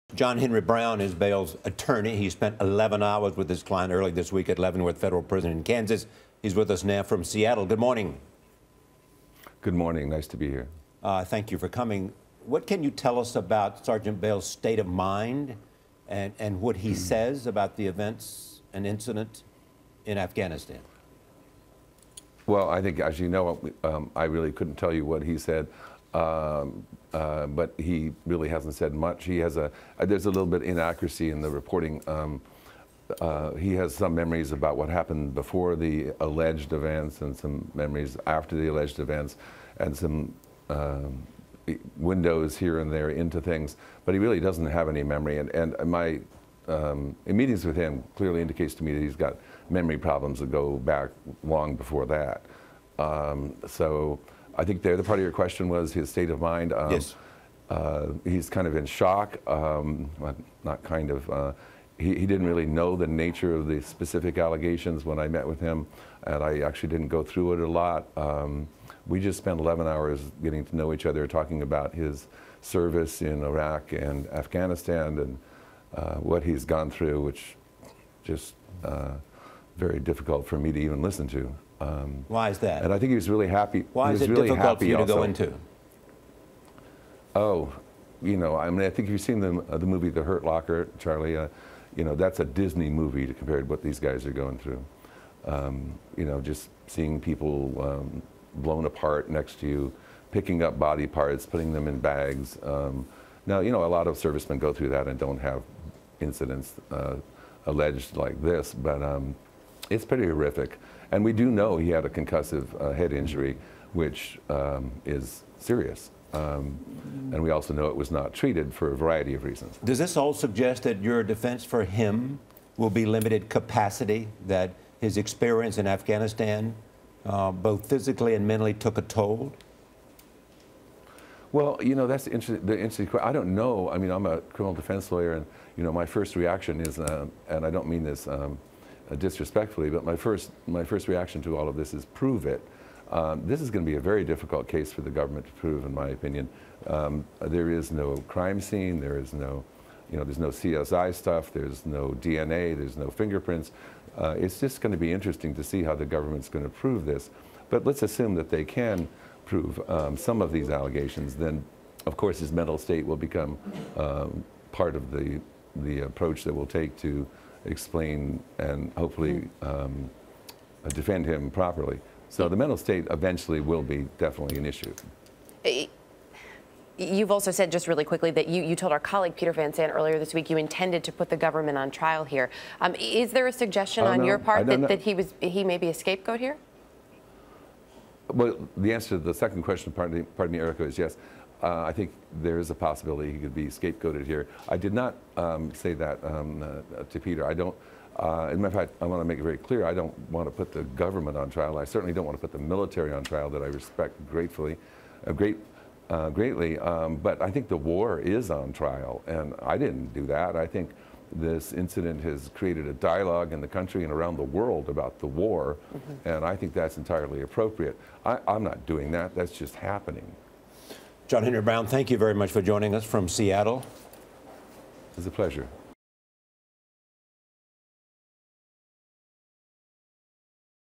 Bales' attorney on charges, meeting client
John Henry Browne, the attorney for Army Staff Sgt. Robert Bales, talks to Charlie Rose and Erica Hill about the charges his client is expected to receive.